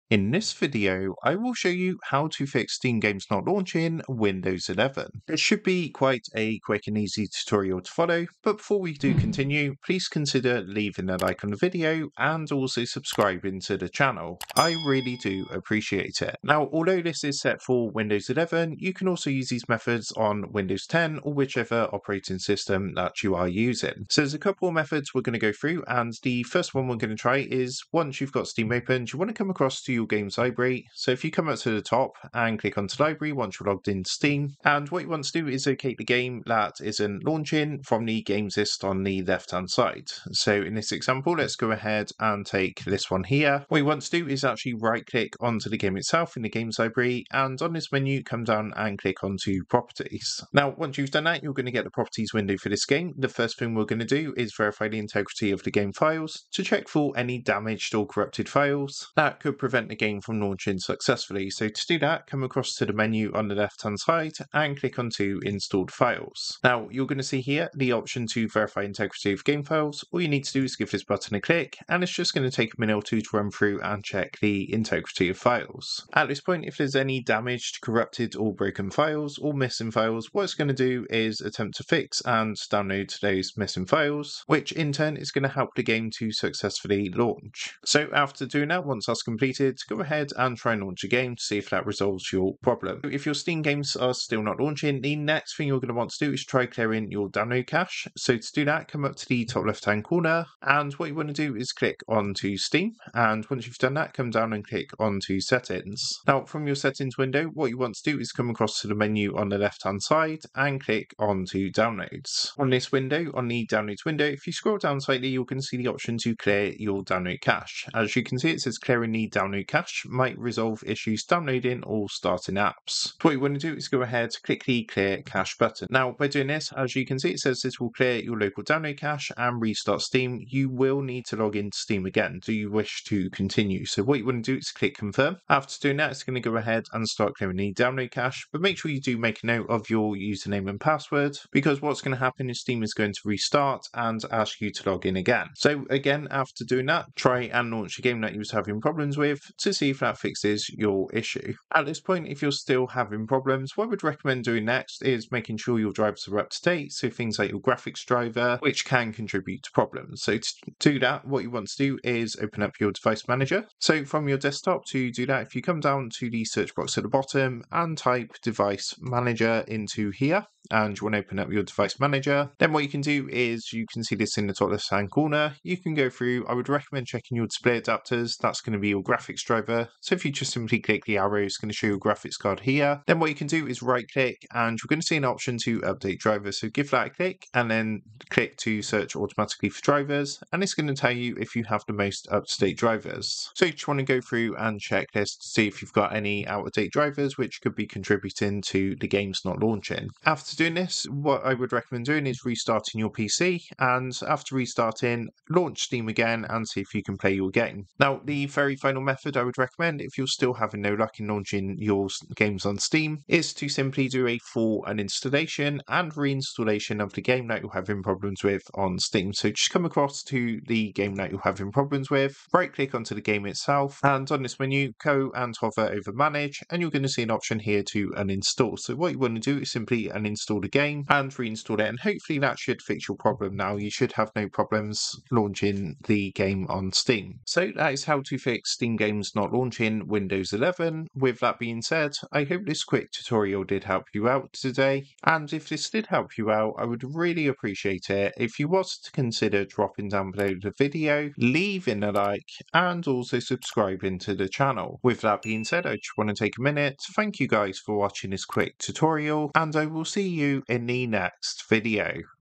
How To Fix Steam Games Not Launching Windows 11
In this video, we tackle the frustrating issue of Steam games not launching on Windows 11. If you're experiencing problems with your favorite games failing to start, you're not alone! Join us as we explore common causes and provide step-by-step solutions to get your games back up and running smoothly. From adjusting your Steam settings to verifying game files and reinstalling Steam, we’ve got you covered with practical tips to ensure a seamless gaming experience. Don’t let technical issues hold you back—let’s fix it together and get you back to gaming in no time!

 ✨Recommended VPN: ✨

Thanks!